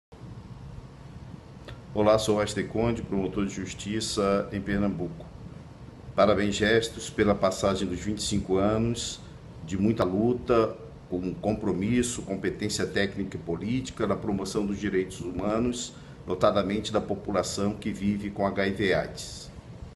Westei Conde y Martin Junior - Promotoria de Justiça de Cidadania da Capital MPPE
Mensagem pelos 25 anos de aniversário da Gestos.